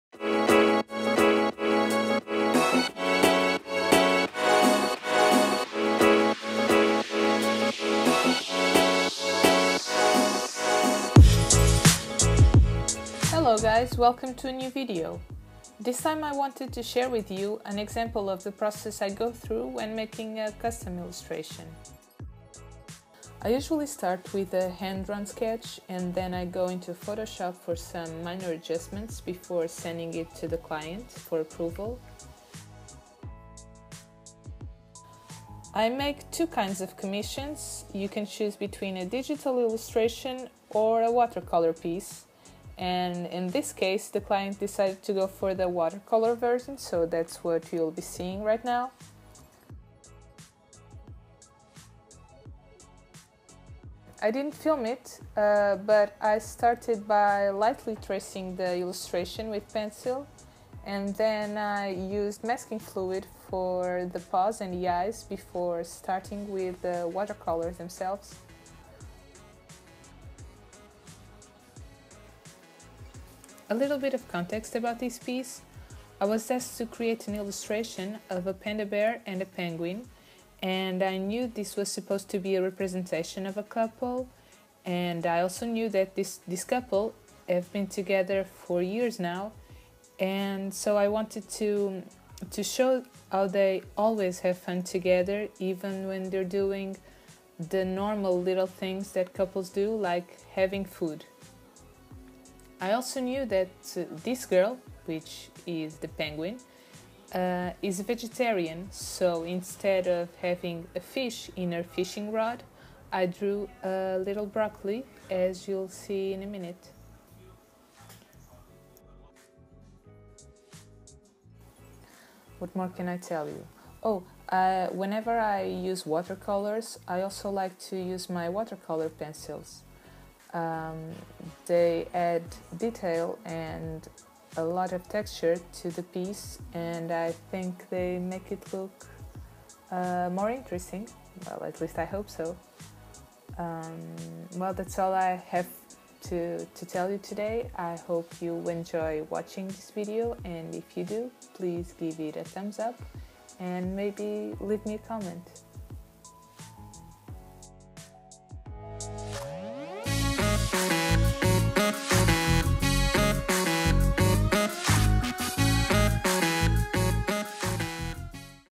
How I Create a Custom illustration
Hello! Today I'm sharing a bit of my process when I create a custom illustration for a client. I hope you like it and if you have any doubts drop me a line!

MY LINKS